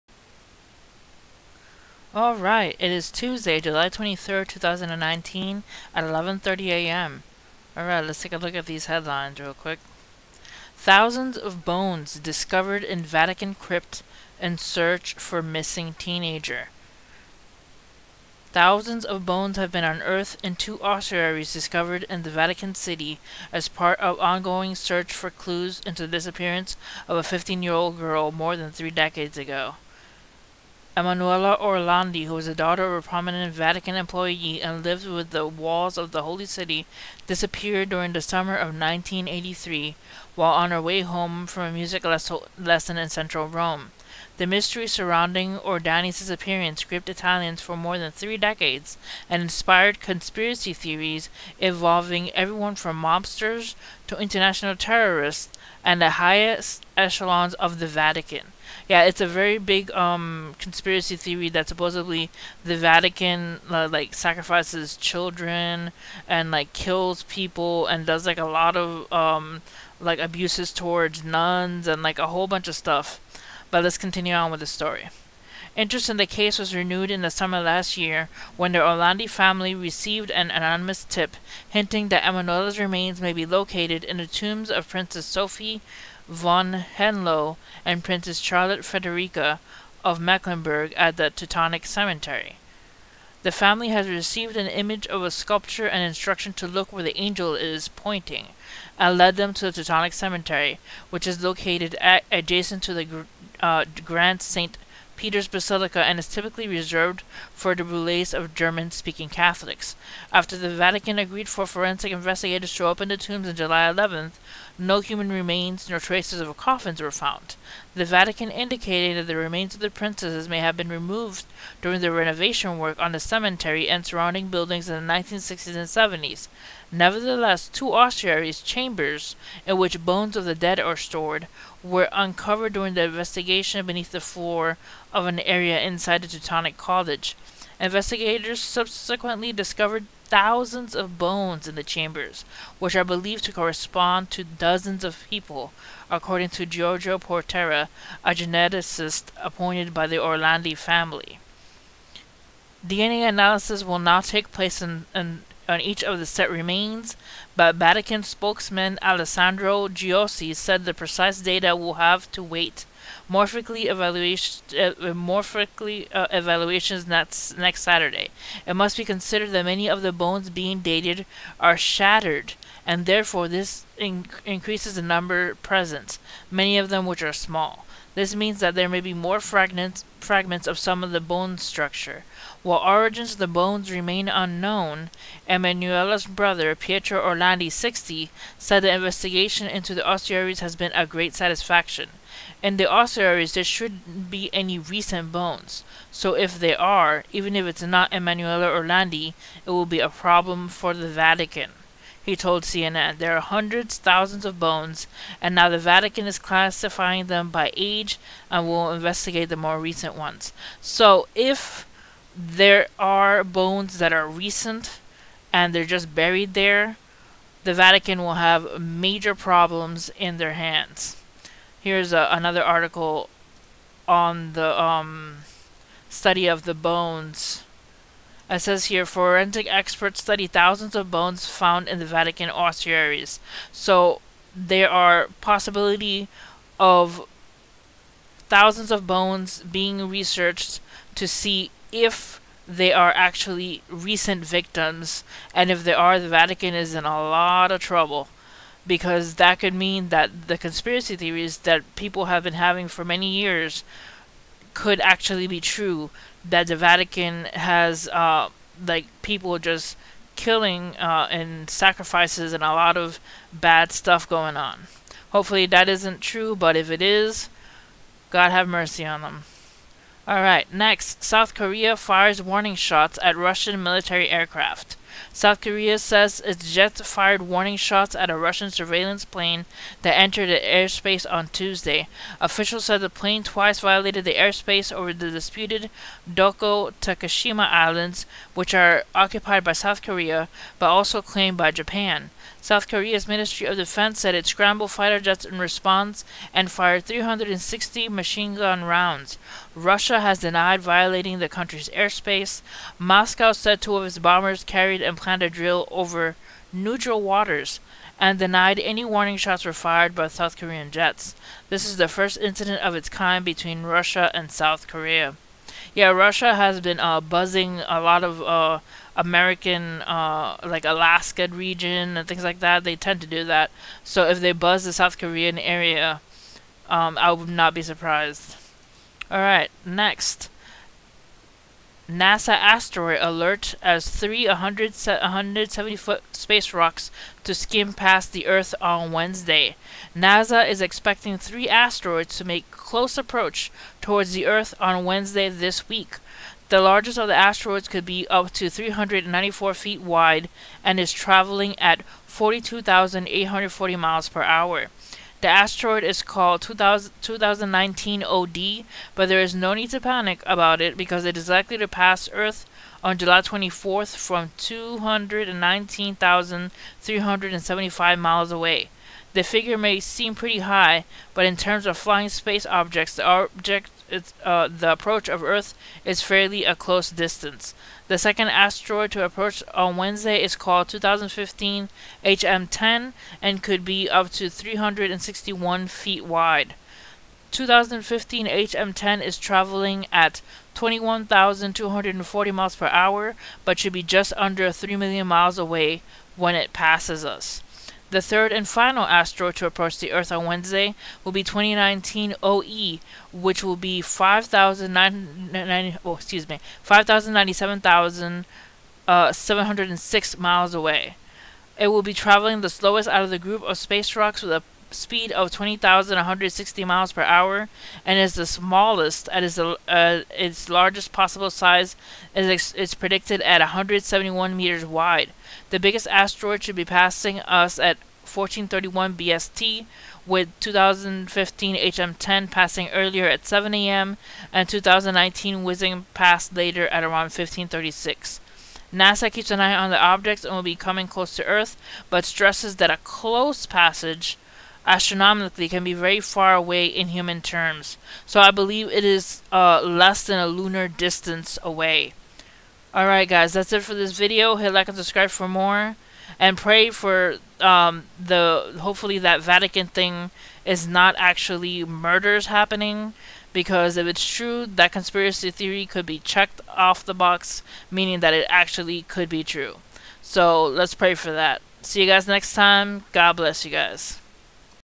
Thousands of bones found in Vatican
Forensic experts to study 'thousands of bones' found in Vatican ossuaries. Thousands of bones discovered in Vatican crypt in search for missing teenager. South Korea fires warning shots at Russian military aircraft. Nasa asteroid alert as THREE 170-foot space rocks to skim past Earth on Wednesday.